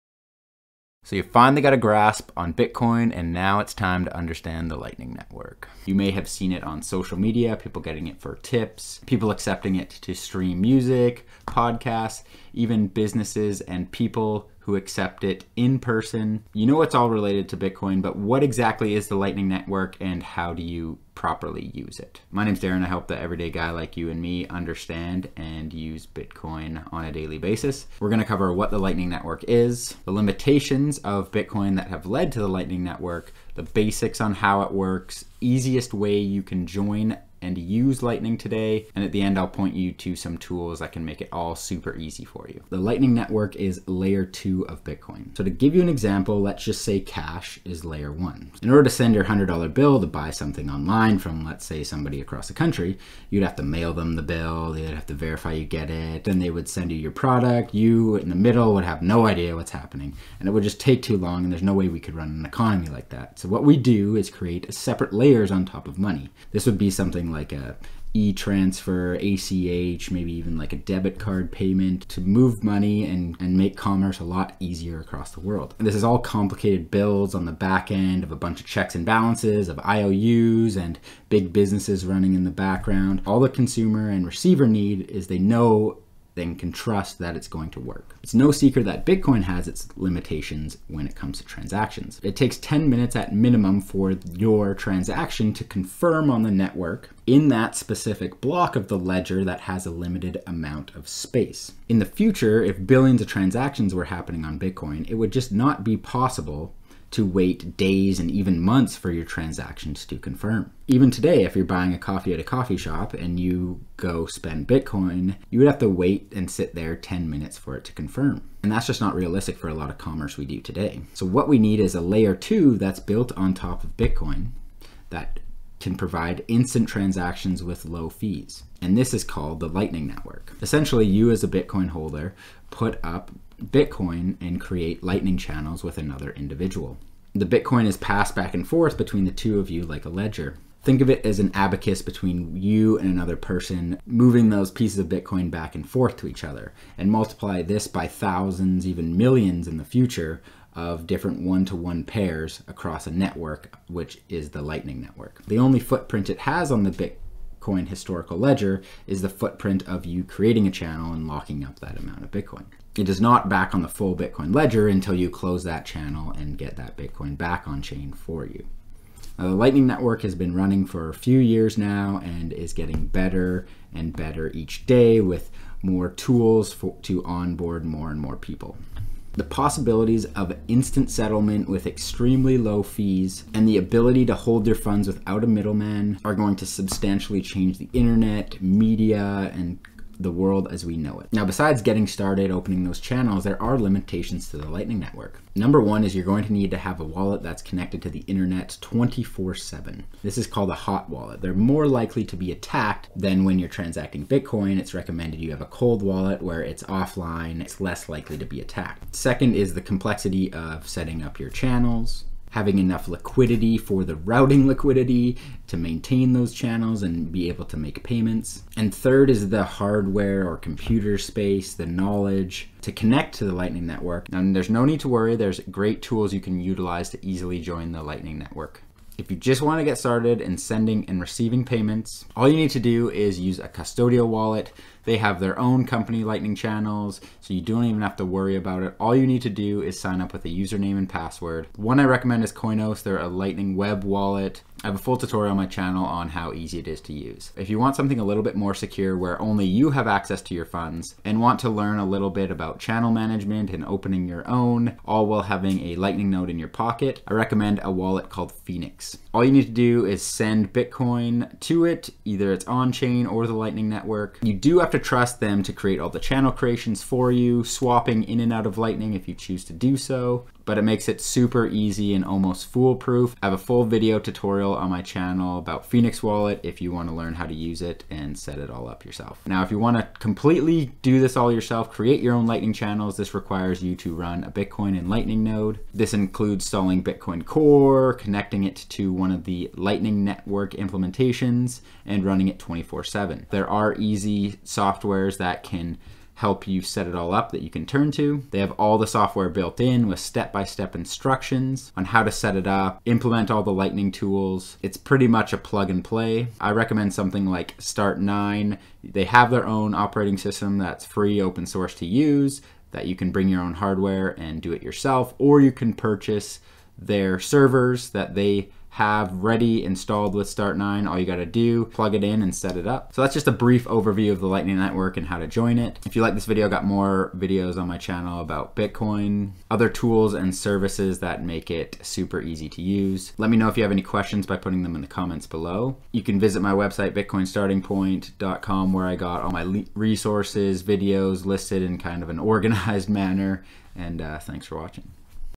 BITCOIN LIGHTNING NETWORK EXPLAINED ⚡ - Easy Guide to Use & Join.
The Lightning Network is the 2nd layer of Bitcoin.    It allows for instant settlement and cheap transactions.

What is the Lightning network

Timestamps:
0:00 Intro
0:43 Layer 2
1:28 Bitcoin Limitations
2:04 Bitcoin Lightning Network
2:49 Growth of Lightning
3:13 Limitations
3:45 Getting Started With Lightning
4:13 Phoenix Wallet
4:49 Running a Lightning Node

bc1qxa4z67pzz7ueasj9c44wlqfptzdzhkfyc8z6ze